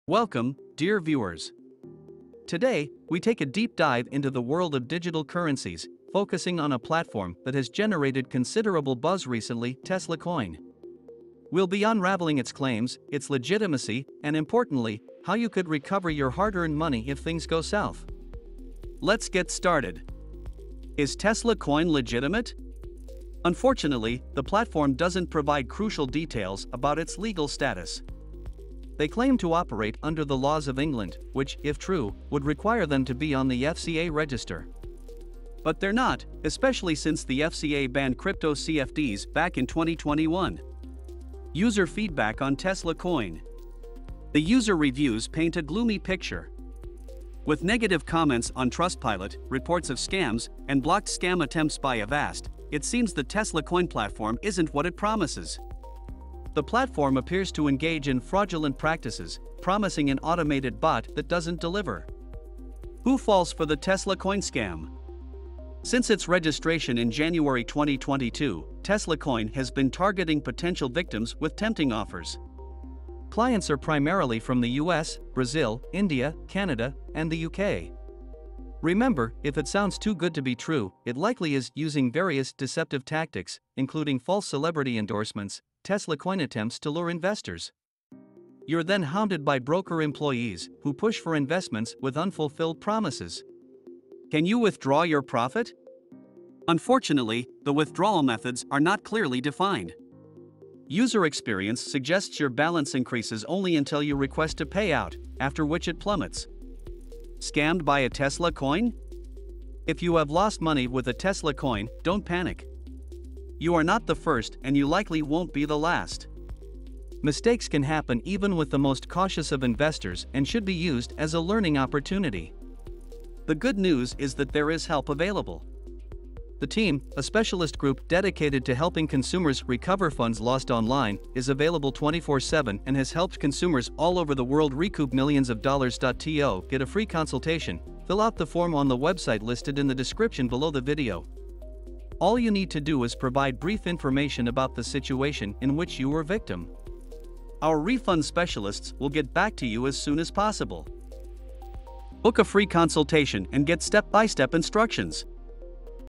Tesla Coin App Scam or Legit? An In-depth Review
Join us as we unravel the truth behind the buzz in this comprehensive 'Tesla Coin Review' 🕵️‍♂️. Is it a revolutionary platform as it claims, or is the 'Tesla Coin Scam' real? Let's decode the 'Tesla Coin App' together!

⏲️TIMESTAMPS⏲️
0:00 - Intro: Understanding Tesla Coin
0:20 - Is Tesla Coin Legitimate? 🔍
0:40 - User Feedback on Tesla Coin 🗣️
1:00 - Who Falls for the Tesla Coin Scam? 😮
1:35 - The Tesla Coin Scheme Unveiled 🚫
1:47 - Trading Platforms and the Tesla Coin App 📲
2:30 - Have You Been Scammed by Tesla Coin? 🆘